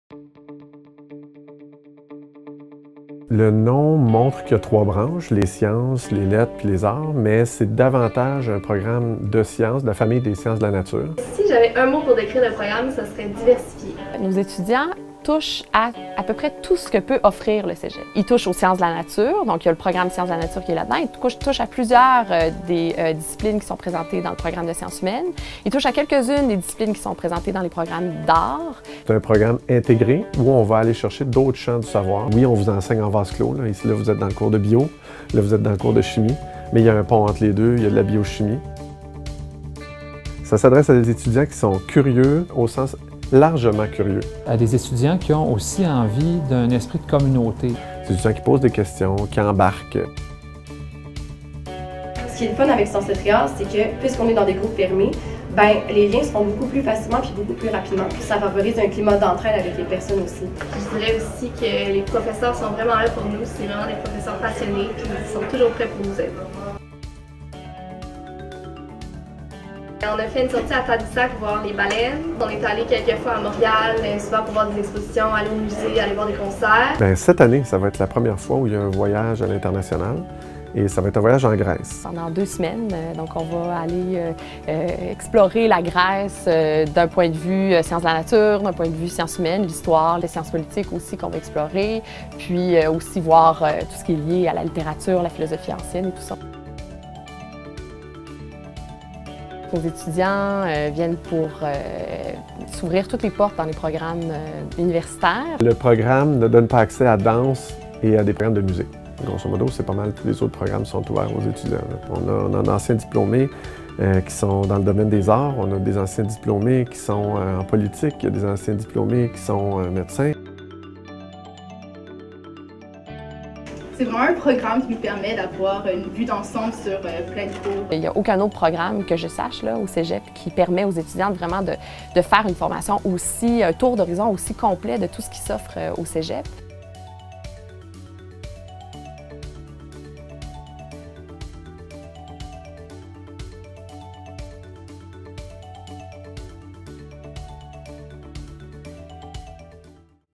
Sciences, lettres et arts - Cégep de Sainte-Foy
Intéressé à la fois par les sciences de la nature, les sciences humaines et les arts ?

Le programme Sciences, lettres et arts vous propose une formation scientifique complète dans un groupe stable qui favorise l'entraide en plus d'acquérir une solide préparation aux études universitaires dans de nombreux domaines. Ces deux années seront riches en rencontres avec de grands penseurs, chercheurs et artistes qui ont marqué notre histoire.